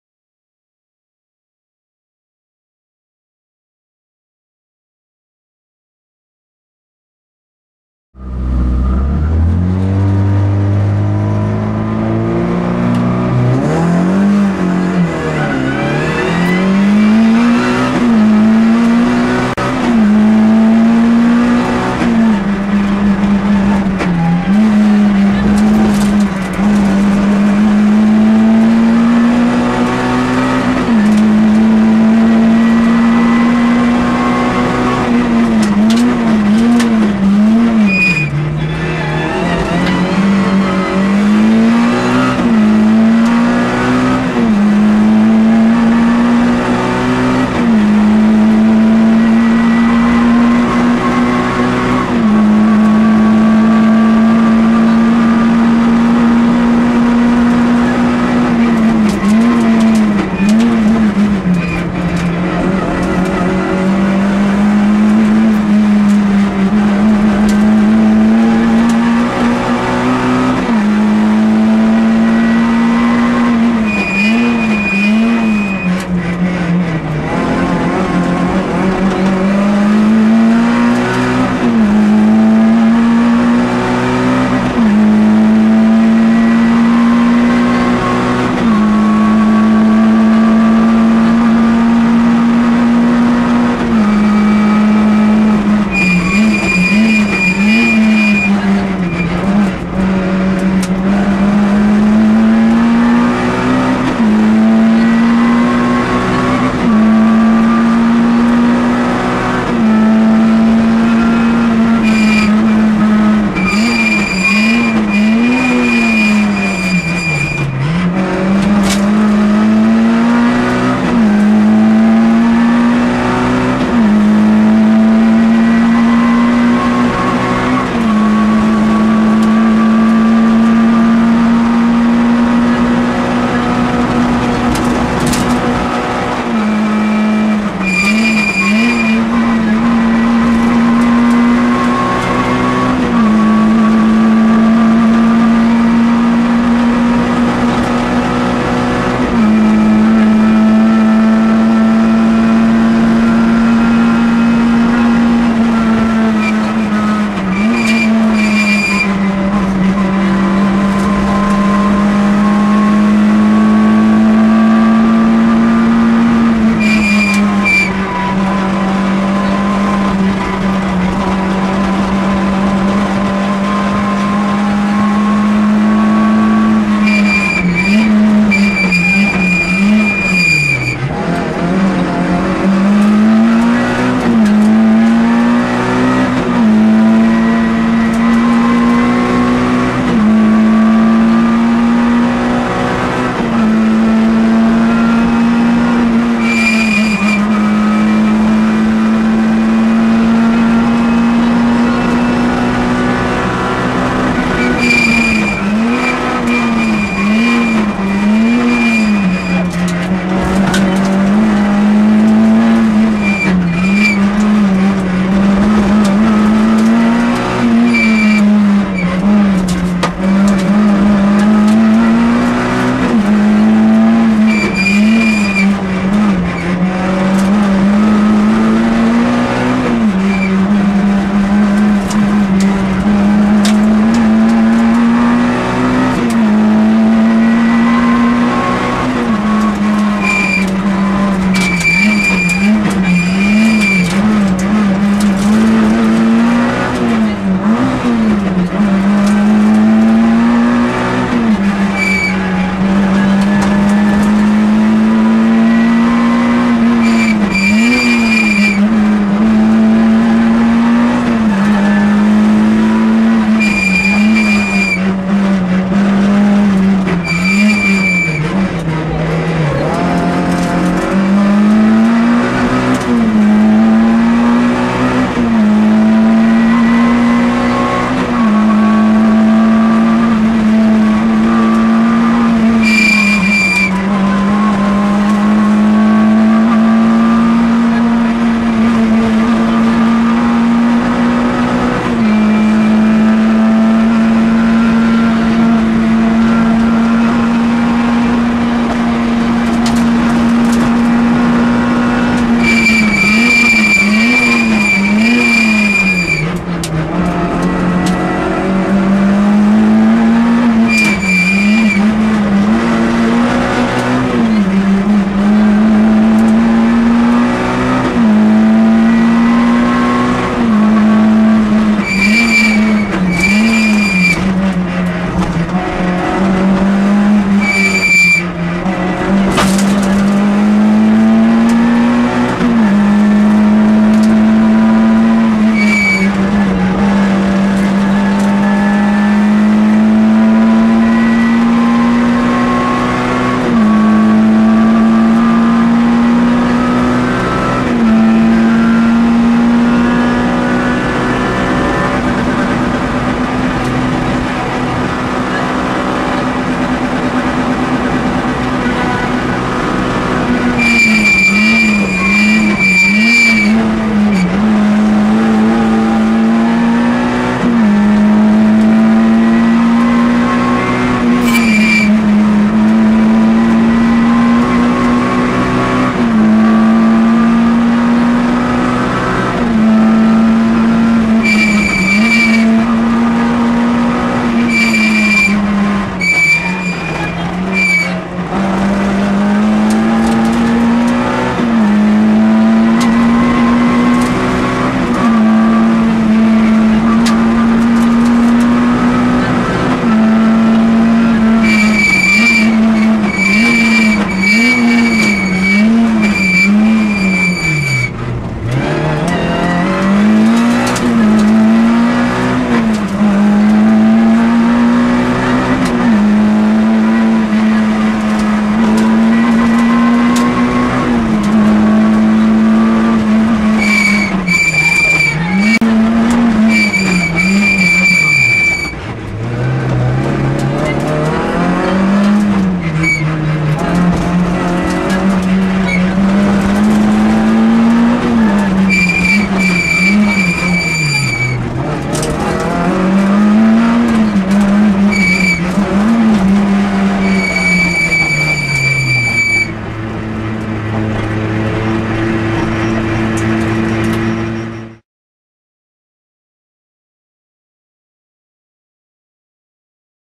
Rallye des Vins-Mâcon 2010 - ES4
Rallye des Vins Mâcon 2010
ES4
Citroën Saxo A6
Lionel Munsch - Amandine Mounard